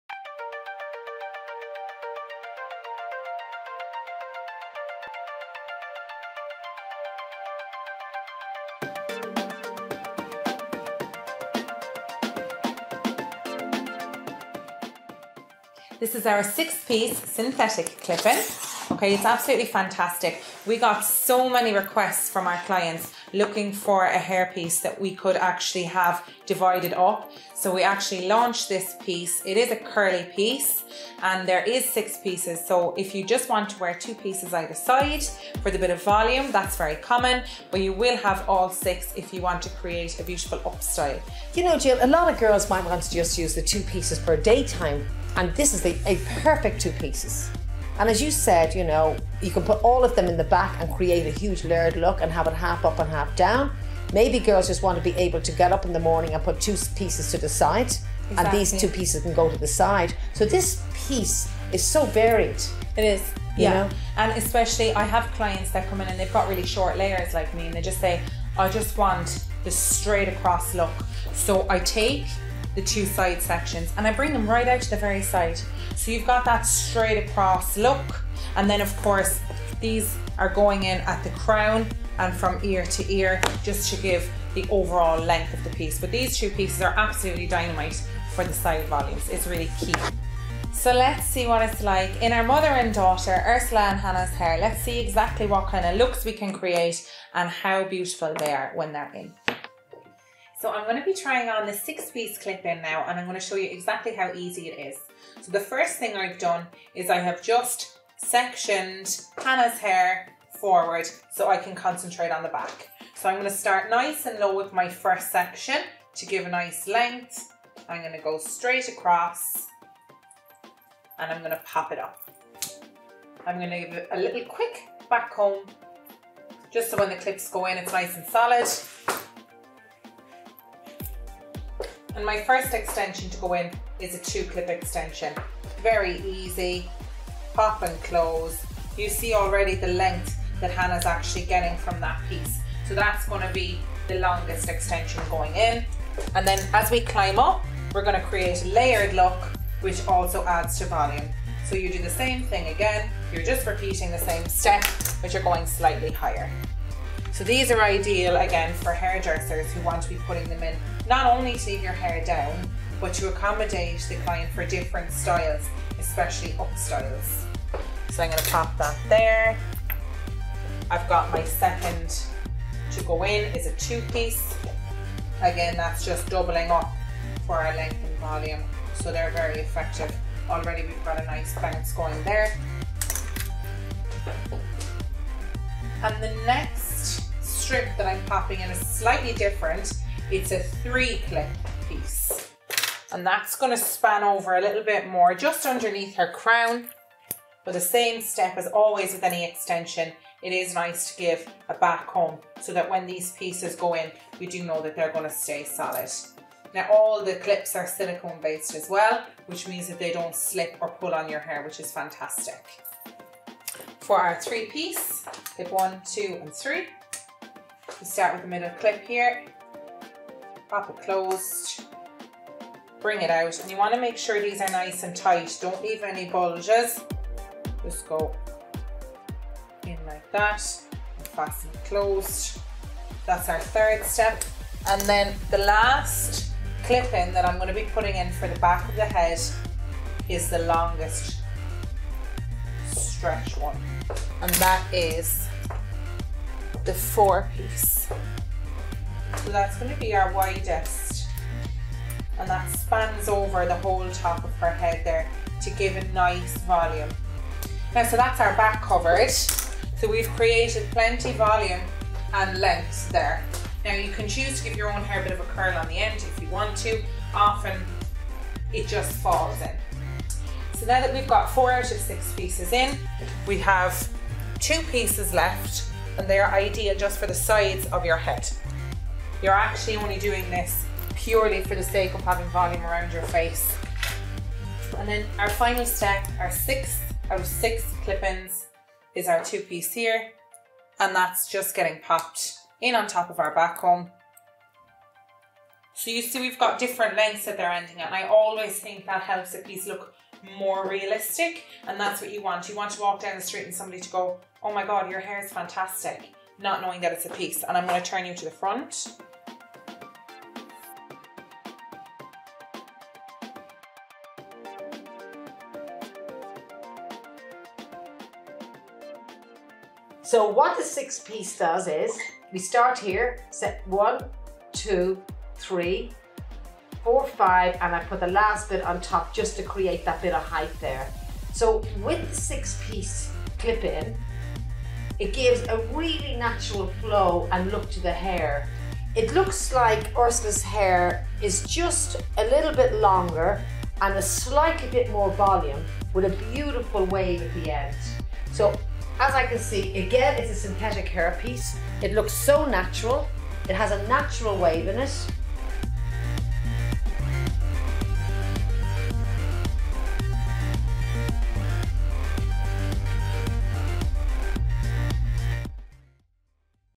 Vanity 6 Piece Clip-in Hair Extension
The 6 piece clip-in hair extension is the perfect versatile clip in that gives instant length and volume. You can buy in any of our hair shops which are in Dublin and Cork.